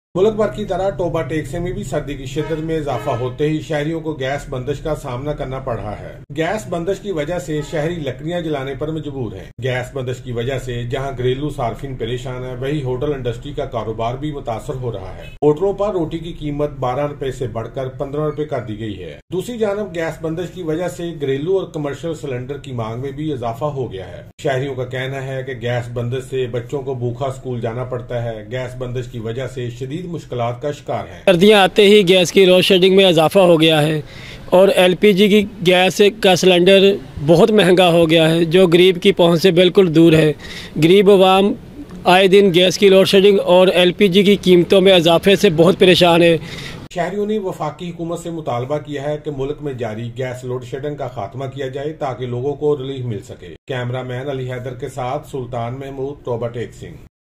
Gas outage in Toba Tek Singh makes life difficult for citizens
PKG
  Increase in demand for LPG gas and firewood due to Toba Tek Singh gas shutdown
In Toba Tek Singh city, bread started selling for fifteen rupees
OC
Like the rest of the country, the gas shutdown in Toba Tek Singh has made it difficult for the citizens to live. The demand for domestic cylinders and firewood has increased enormously, while the price of bread has also reached 15 rupees. Sultan Sidhu's report.
VO
Like the rest of the country, the citizens of Toba Tek Singh are facing gas cut as the cold weather increases
Citizens are forced to burn firewood due to gas shutdown
  While the domestic consumers are worried due to the gas shutdown, the business of the hotel industry is also affected
The price of bread at hotels will be increased from 12 rupees to 15 rupees.
On the other hand, the demand for domestic and commercial gas cylinders has also increased due to gas shutdown
Citizens say that children have to go to school hungry due to gas shutdown.
Seven. Citizens
The citizens have demanded the federal government to end the ongoing gas loadshedding in the country so that people can get relief

Footage
Sultan Sidhu Toba Tek Singh with Cameraman Rashid Anjum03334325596#TOBATEKSINGHNEWS